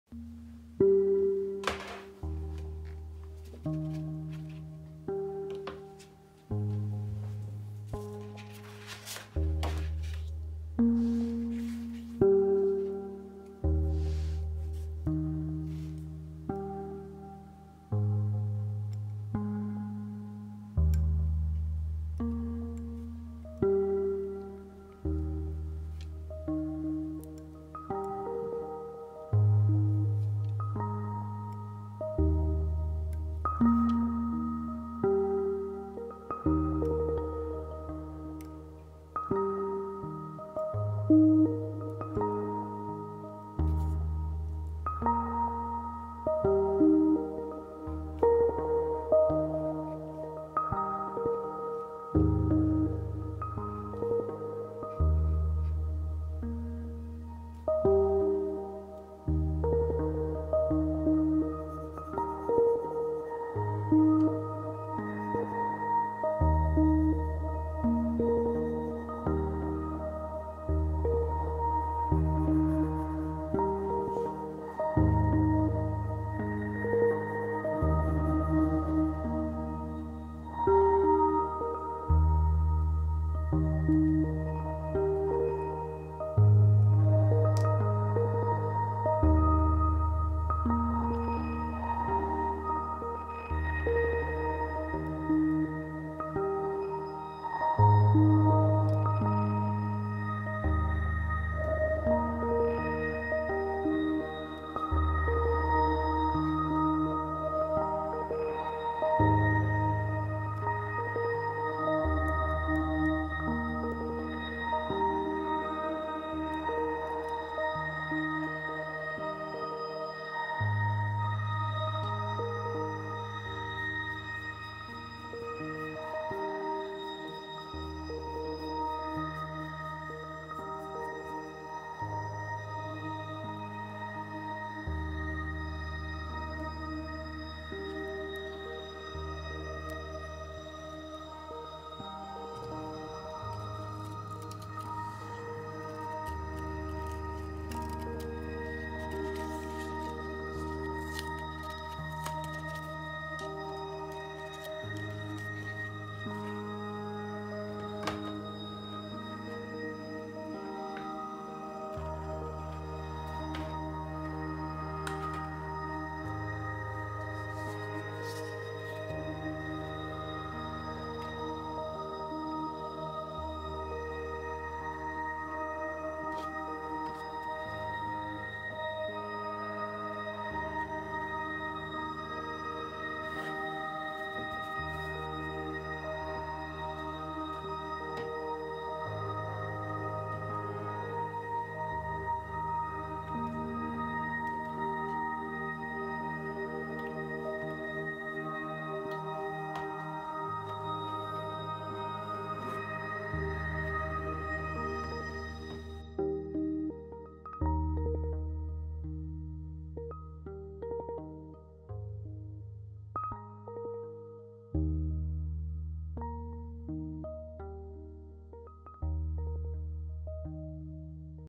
PAPERCUTTING｜Proceso de trabajo
En este video muestro cómo hice un trabajo con la técnica de papercutting (o calado). Me tomó unas 2 horas.